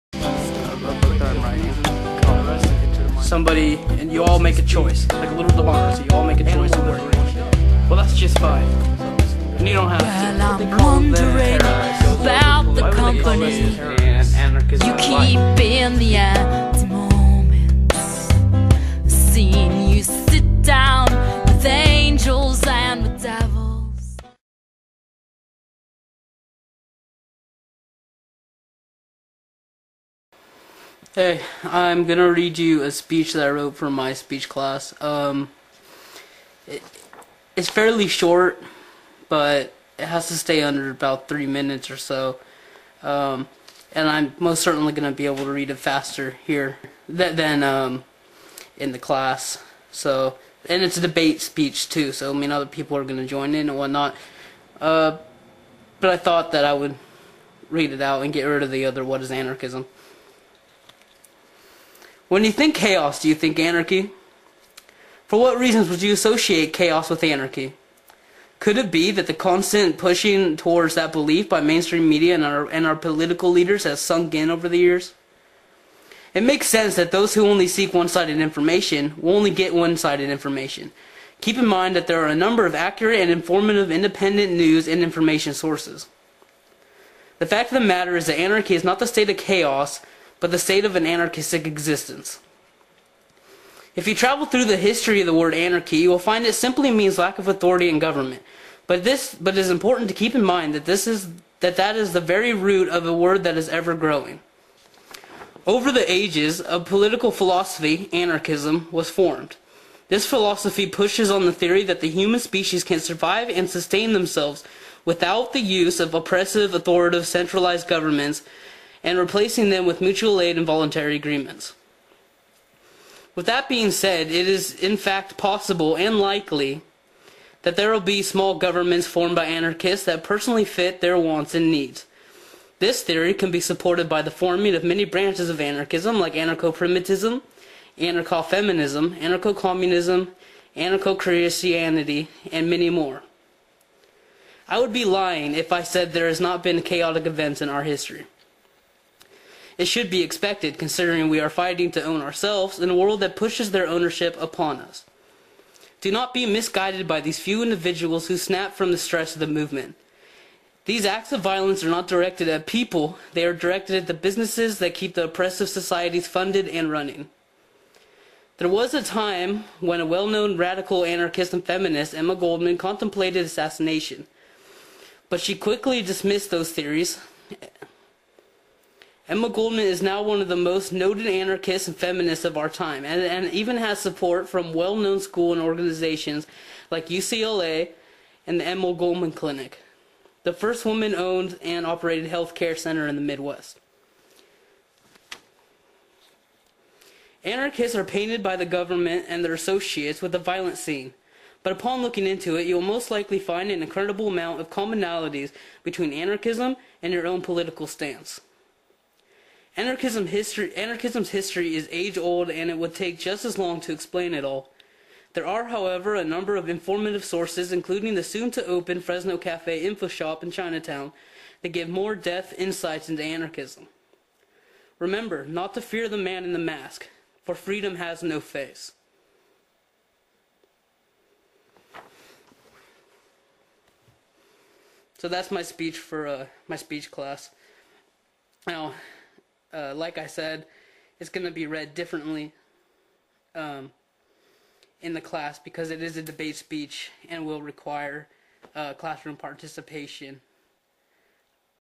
A.C. Info - Anarchism In Short
I prefer a little strech over black lines. Writen Speech :

Anarchism In Short

When you think chaos do you think anarchy? For what reasons would you associate chaos with anarchy? Could it be that the constant pushing towards that belief by mainstream media and our political leaders has sunk in over the years? It makes sense that those who only seek one sided information will only get one sided information, keep in mind there are a number of accurate and informative independent news and information sources. The fact of the matter is that anarchy is not the state of chaos, but the state of an anarchist existence.

If you travel through the history of the word anarchy you will find it simply means lack of authority and government, but it is important to keep in mind that is the very root of a word that is ever growing.

Over the ages a political philosophy, anarchism, was formed. This philosophy pushes on the theory that the human species can survive and sustain themselves without the use of oppressive, authoritative centralized governments and replacing them with mutual aid and voluntary agreements. With that being said it is in fact possible, and likely, that there would be small governments formed by anarchists that personally fit their wants and needs. This theory is supported by the forming of many branches of anarchism, like Anarcho-Primitivism, Anarcha-Feminism, Anarcho-Communism, Anarcho-Christianity and many more.

I would be lying if I said there has not being chaotic events in our history. It should be expected, considering we are fighting to own ourselves in a world that pushes their ownership upon us. Do not be misguided by these few individuals who snap from the stress of the movement, these acts of violence are not directed at people they are directed at the businesses that keep oppressive society's funded and running. There was a time when the well known radical anarchist and feminist Emma Goldman contemplated assassination, but she quickly dismissed those theories. Emma Goldman is now one of the most noted anarchists and feminist of our time and even has support from from well known school and organizations like UCLA and the Emma Goldman Clinic, The first women owned and operated health care center in the Midwest.

Anarchists are painted by the government and their associates with a violent scene, but upon looking into it you will most likely find an incredible amount of commonalities between anarchism and your own political stance. Anarchisms history is age old and it would take just as long to explain it all. There are however a number of informative sources, including the soon to open Fresno Caffe Info shop in Chinatown, that give more depth insights into anarchism. Remember not to fear the man in the mask, for freedom has no face